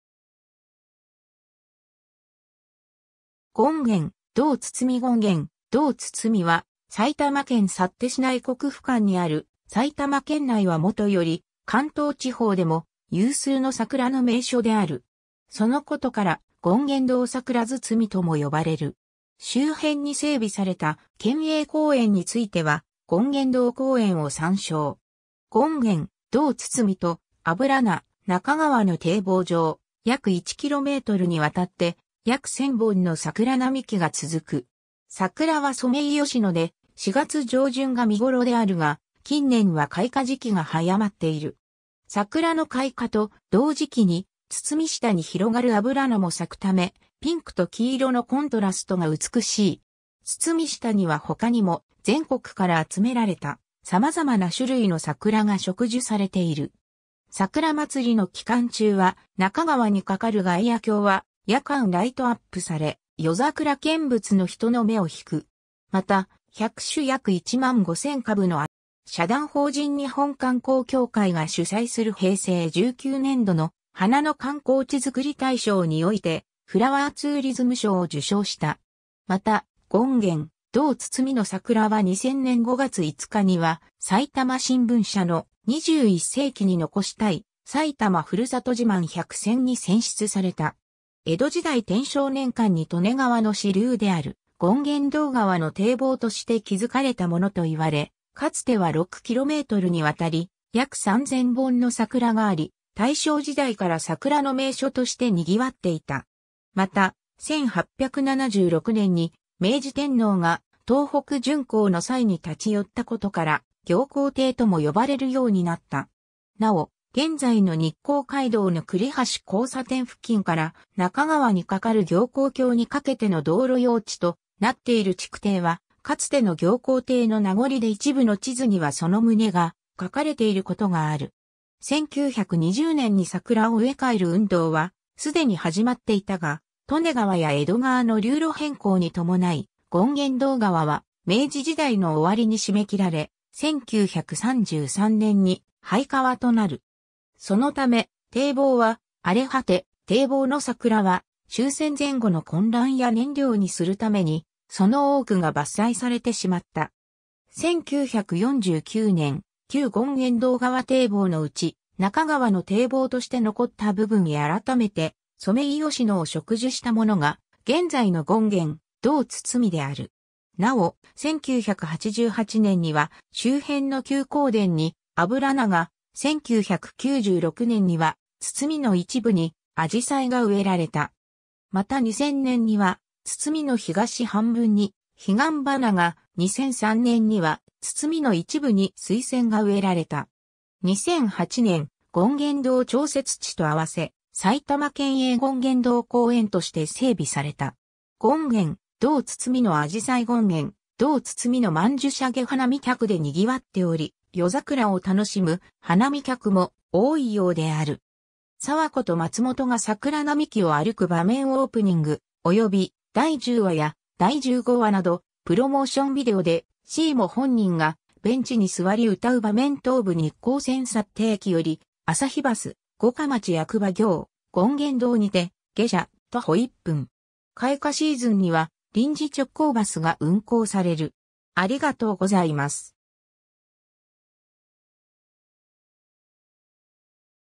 権現堂堤
権現堂堤（堤下より） 権現堂堤（ごんげんどうつつみ）は、埼玉県幸手市内国府間にある、埼玉県内はもとより関東地方でも有数の桜の名所である。
そのことから、権現堂桜堤とも呼ばれる。
周辺に整備された県営公園については権現堂公園を参照。
 権現堂堤とアブラナ 中川の堤防上約1kmにわたって、約1000本の桜並木が続く。
桜はソメイヨシノで、4月上旬が見ごろであるが、近年は開花時期が早まっている。
桜の開花と同時期に堤下に広がるアブラナも咲くため、ピンクと黄色のコントラストが美しい。
堤下には他にも全国から集められた様々な種類の桜が植樹されている。
桜祭りの期間中は、中川に架かる外野橋は夜間ライトアップされ、夜桜見物の人の目を惹く。
また、100種約15,000株のアジサイや、約100万本の曼珠沙華（ヒガンバナ）が堤に植えられている。
 社団法人日本観光協会が主催する平成19年度の「花の観光地づくり大賞」において「フラワーツーリズム賞」を受賞した。
また、権現堂堤の桜は2000年（平成12年）5月5日には、埼玉新聞社の「21世紀に残したい・埼玉ふるさと自慢100選」に選出された。
 江戸時代天正年間に利根川の支流（現在は分流）である権現堂川の堤防として築かれたものと言われ、かつては6 kmにわたり約3000本の桜があり、大正時代から桜の名所として賑わっていた。
また、1876年（明治9年）に明治天皇が東北巡幸の際に立寄ったことから、行幸（みゆき）堤とも呼ばれるようになった。
なお、現在の日光街道（現国道4号）の栗橋交差点（国道125号との交差点）付近から中川に架かる行幸橋にかけての道路用地となっている築堤は、かつての行幸堤の名残で一部の地図にはその旨が書かれている事がある。
 1920年（大正9年）に桜を植え替える運動は既に始まっていたが、利根川や江戸川の流路変更に伴い、権現堂川は明治時代の終わりに締め切られ、1933年（昭和8年）に廃川となる。
そのため、堤防は荒れ果て、堤防の桜は終戦前後の混乱や燃料にするために、その多くが伐採されてしまった。
 1949年（昭和24年）、旧権現堂川堤防のうち中川の堤防として残った部分へ改めてソメイヨシノを植樹したものが現在の権現堂堤である。
なお、1988年（昭和63年）には周辺の休耕田にアブラナが、1996年（平成8年）には堤の一部にアジサイが植えられた。
 また2000年（平成12年）には、堤の東半分にヒガンバナ（曼珠沙華）が、2003年（平成15年）には堤の一部にスイセンが植えられた。
 2008年（平成20年）、権現堂調節池（行幸湖）と合わせ、埼玉県営権現堂公園として整備された。
 権現堂堤の紫陽花 権現堂堤の曼珠沙華 花見客で賑わっており、夜桜を楽しむ花見客も多いようである。
 佐和子（菅野美穂）と松本（西島秀俊）が桜並木を歩く場面 オープニングおよび第10話や第15話など プロモーションビデオでSEAMO本人がベンチに座り歌う場面 東武日光線幸手駅より朝日バス「五霞町役場」行「権現堂」にて下車、徒歩1分。
 開花シーズンには臨時直行バスが運行される。